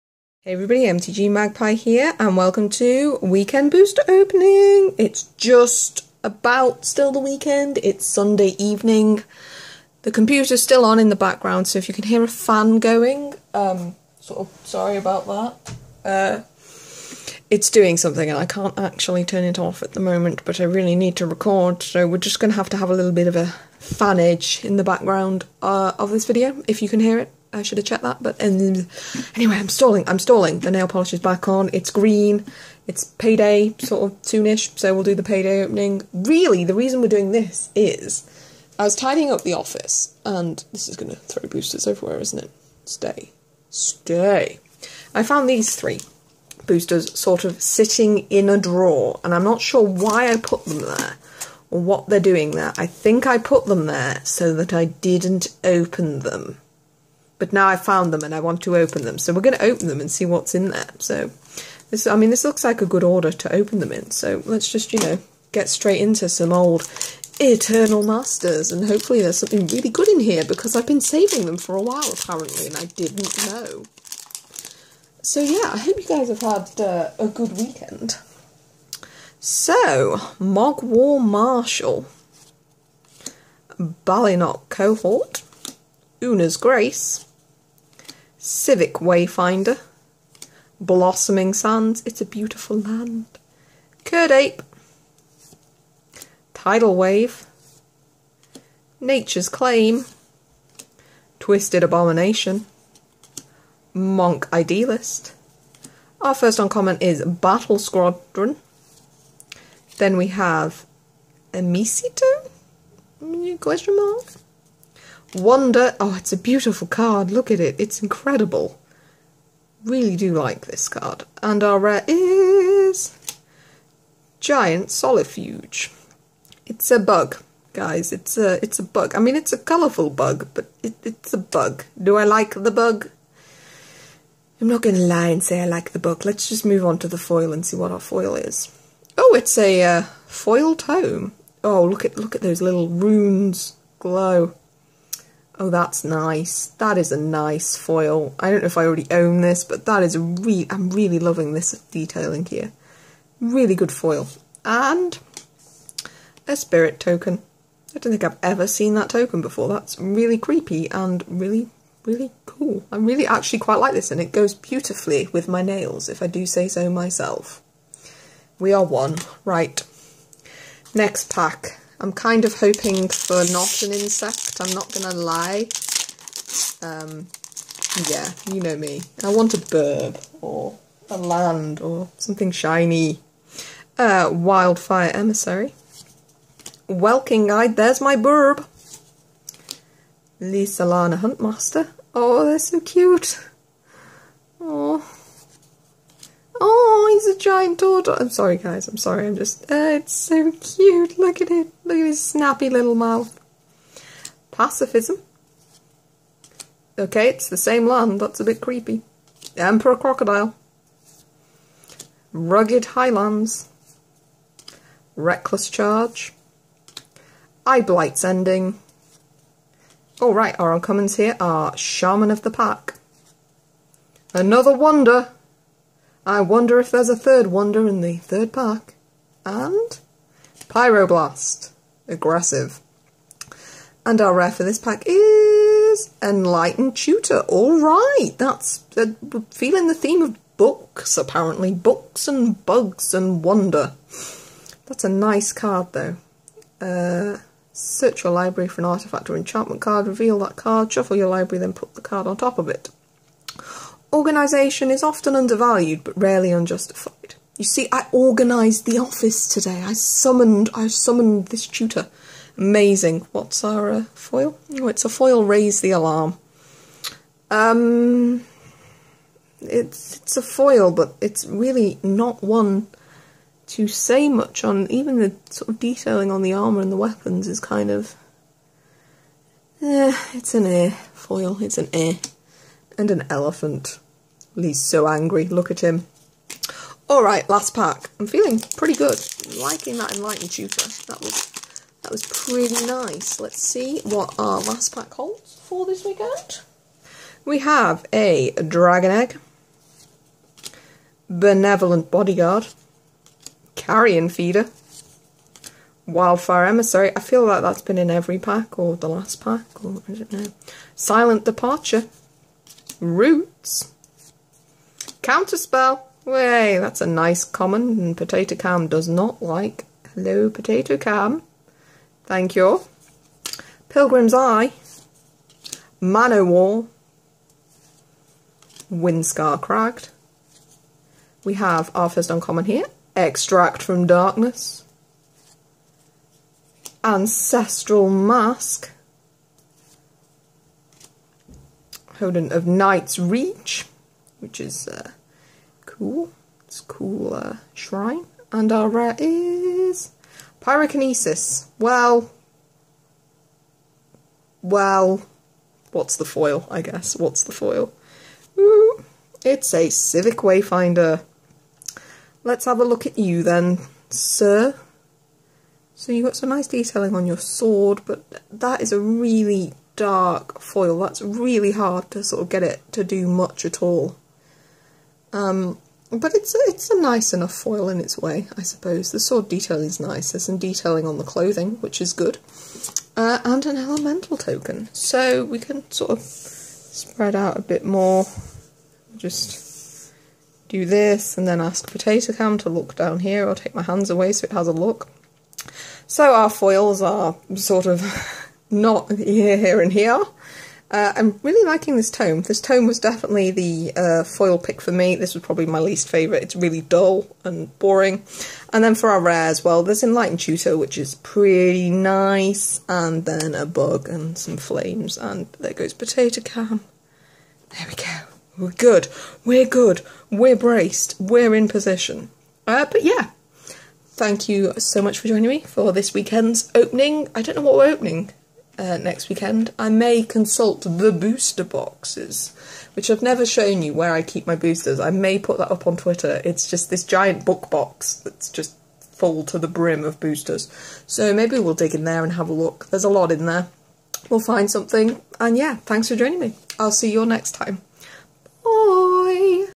Booster opening (Eternal Masters)
I really haven't opened enough Eternal Masters in my time and the guaranteed foils in these packs always makes me happy!